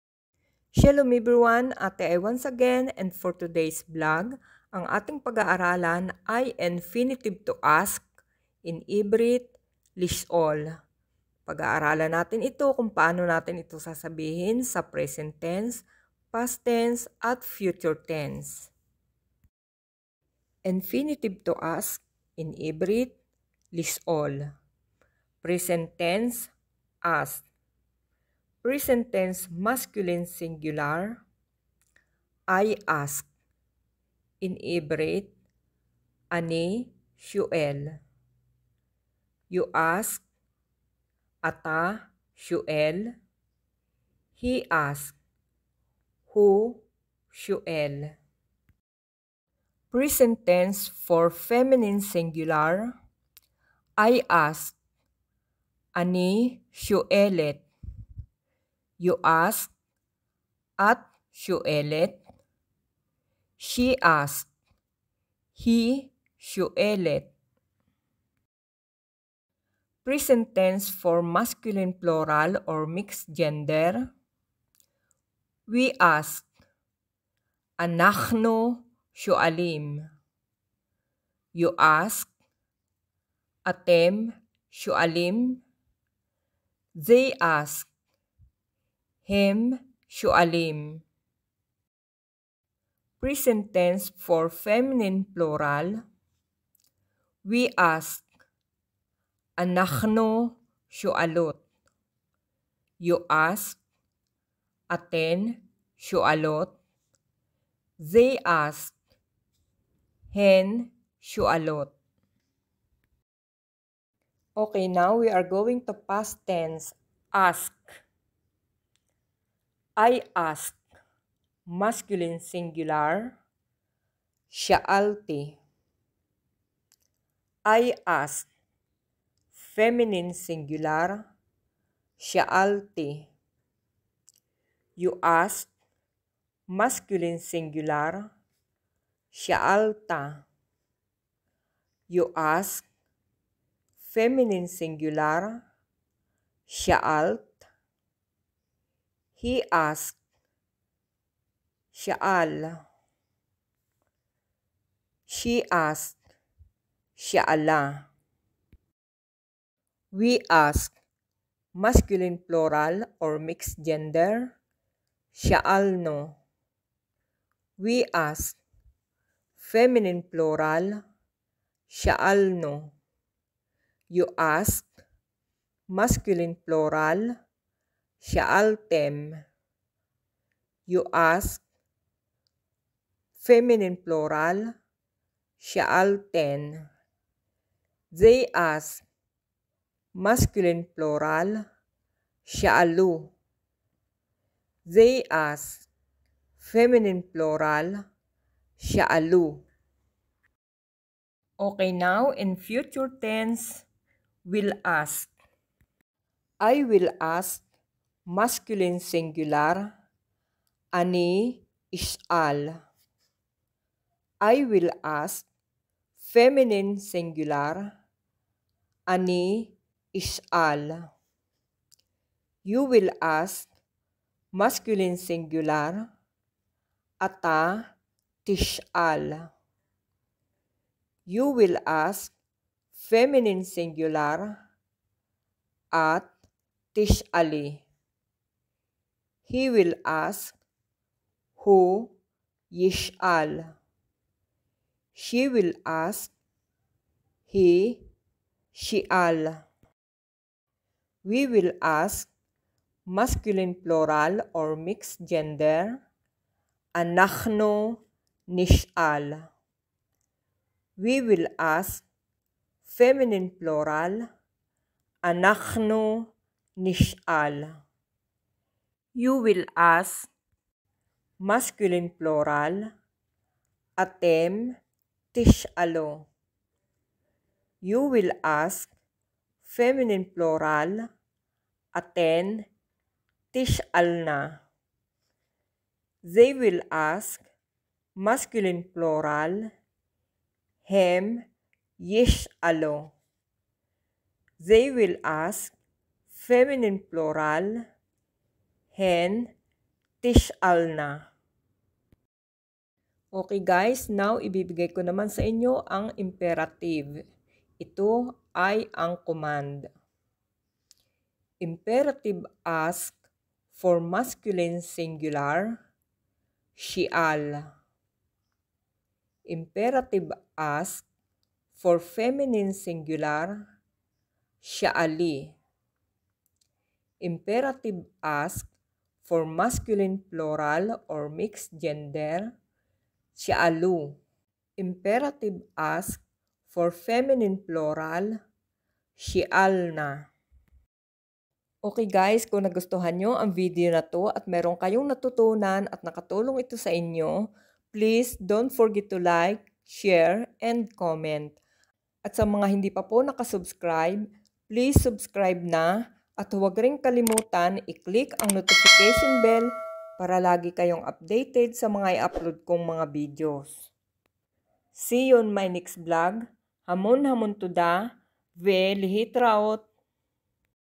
TO ASK - LISH'OL PRESENT, PAST AND FUTURE TENSE
This channel will teach basic hebrew language fir beginner.
All content provided by this channel is intended fir educational only.

This video i'm sharing you guys
the conjugation of verb to ask.